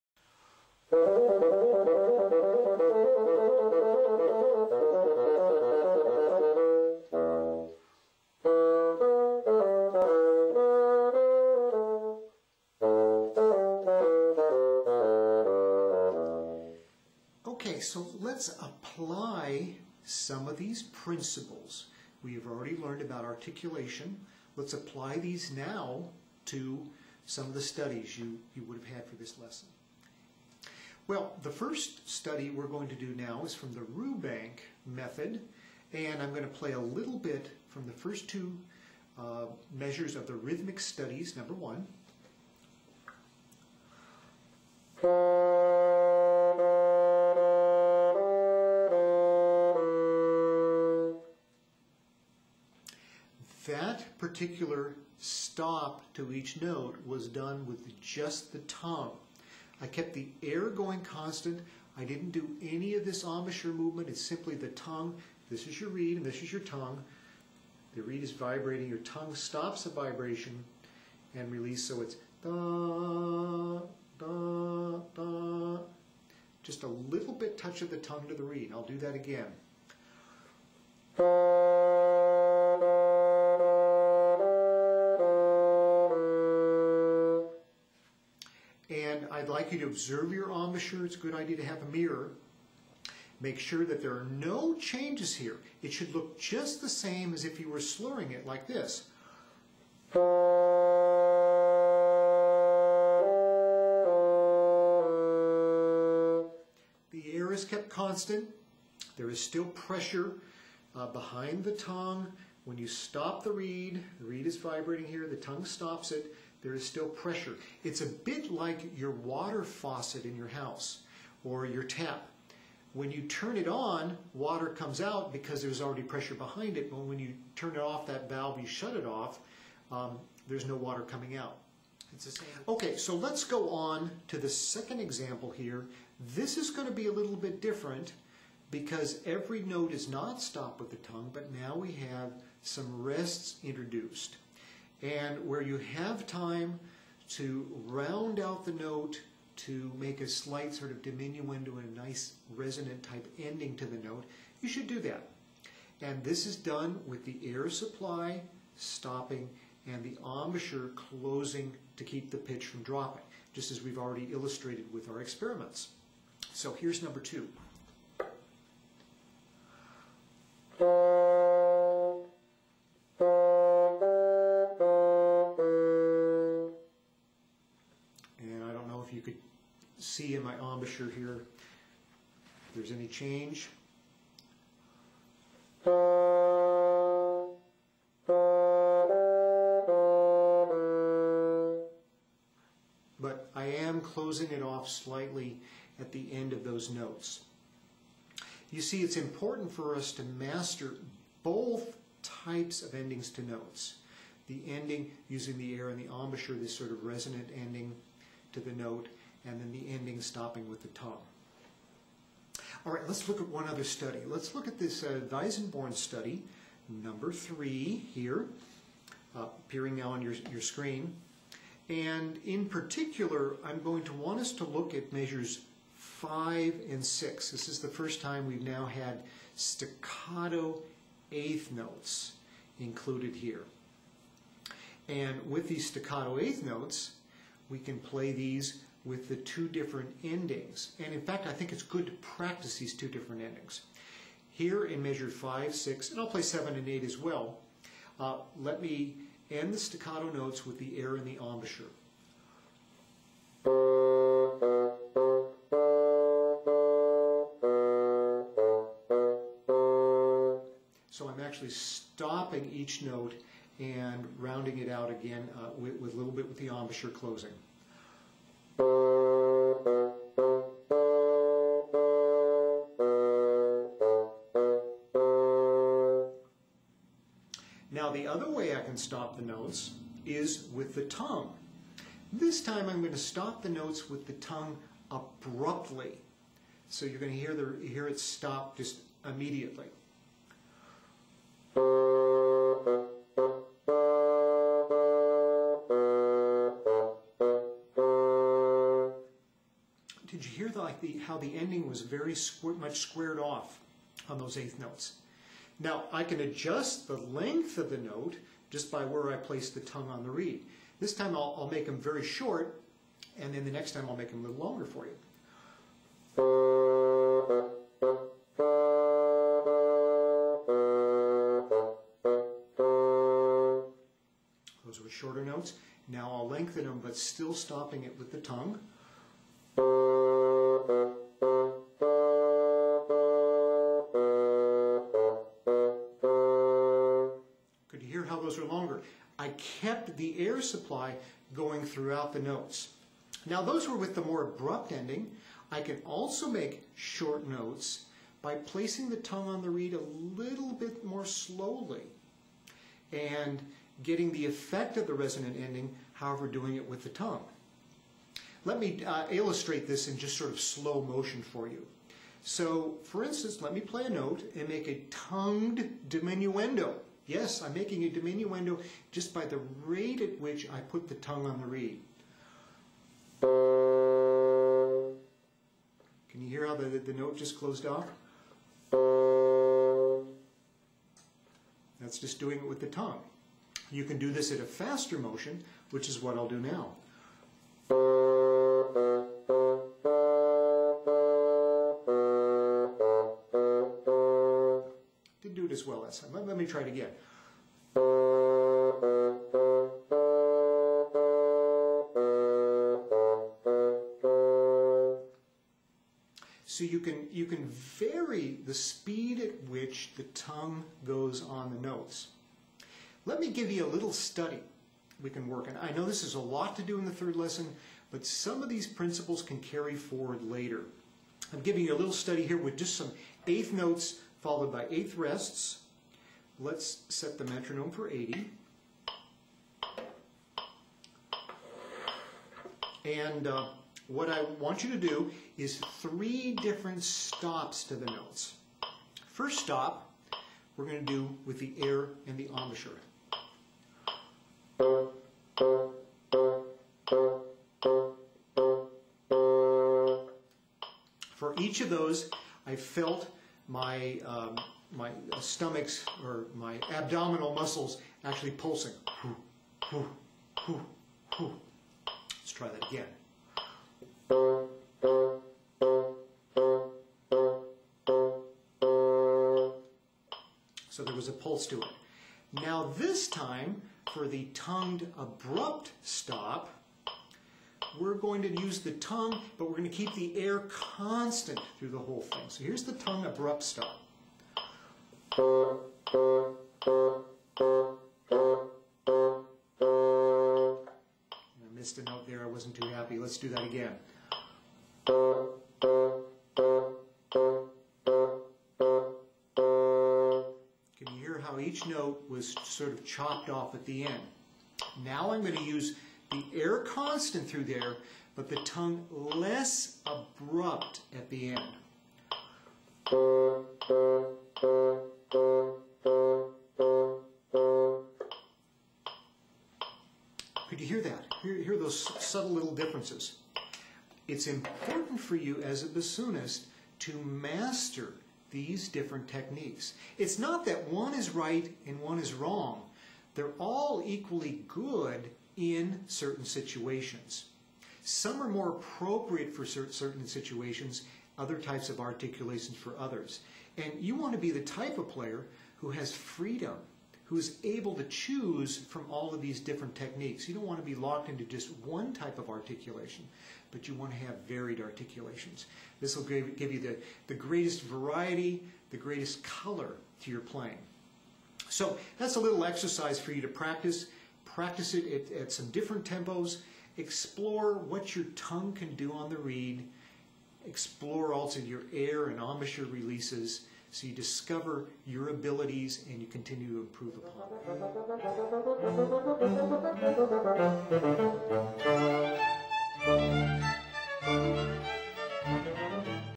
Third Bassoon Lesson: Part 2
Part 2 of the third bassoon lesson with Terry B. Ewell. Three different ways to stop notes. Demonstrations in studies. Bassoon Digital Professor video #34.